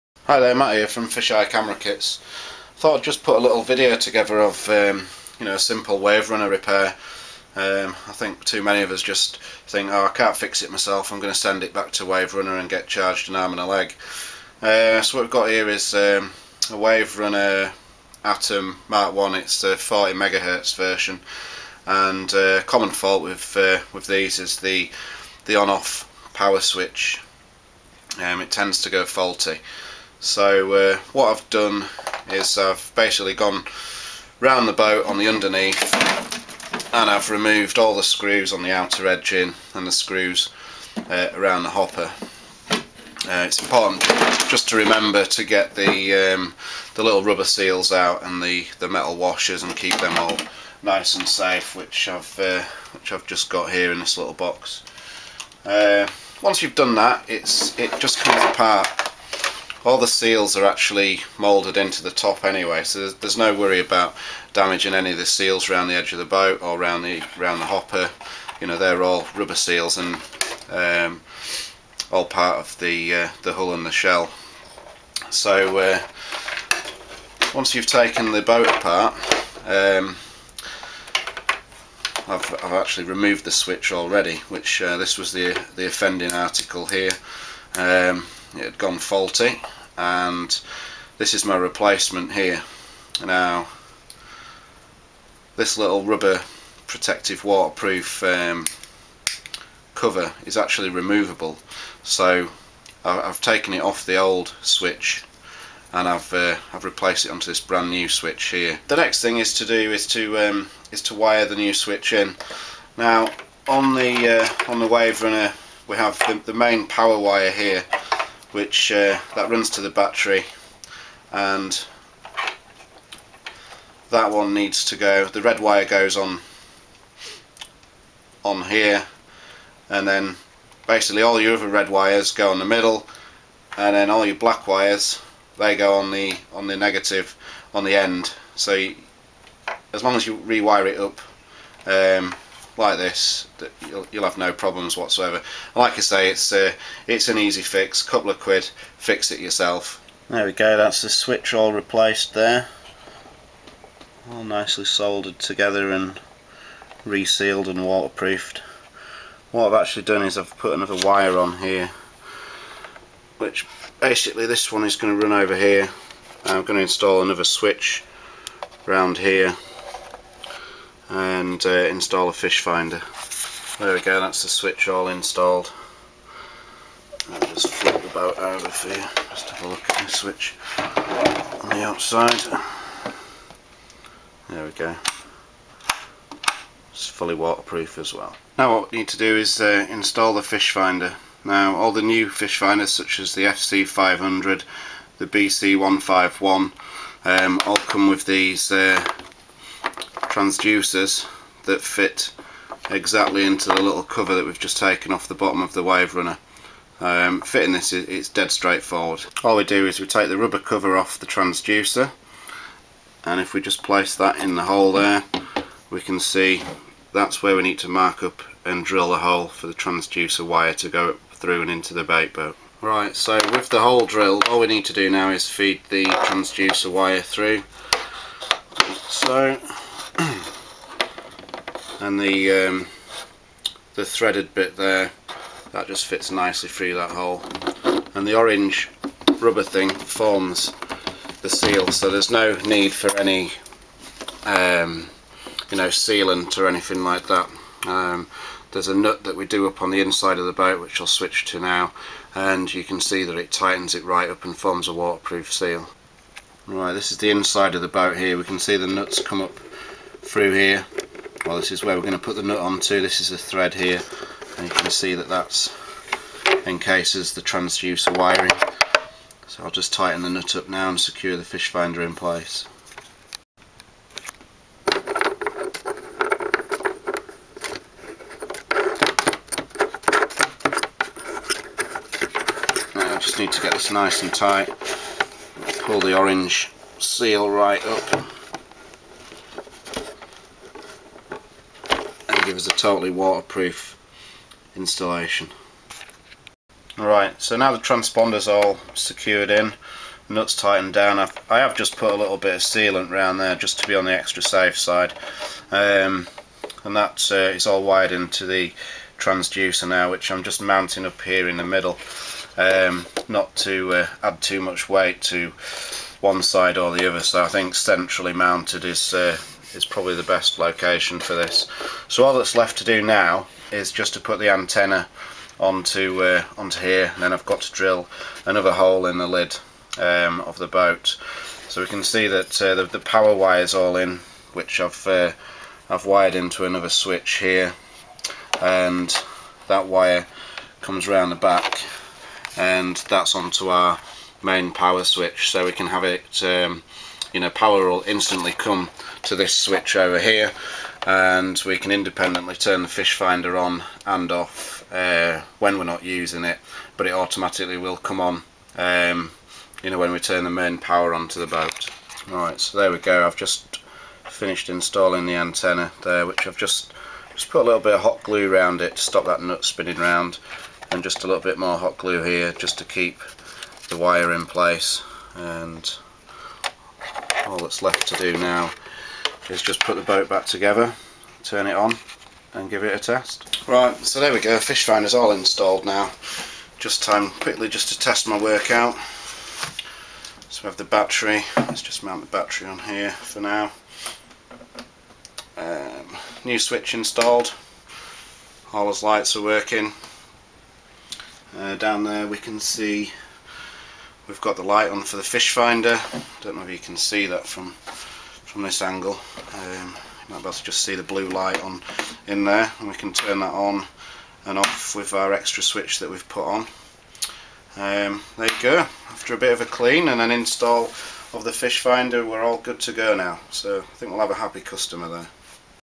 Waverunner Bait Boat Power Switch Replacement FC500 BC151 Fish Finder Installation
A short video on how to repair a faulty Waverunner ON/Off power switch. The video also shows the fitting of a fish finder.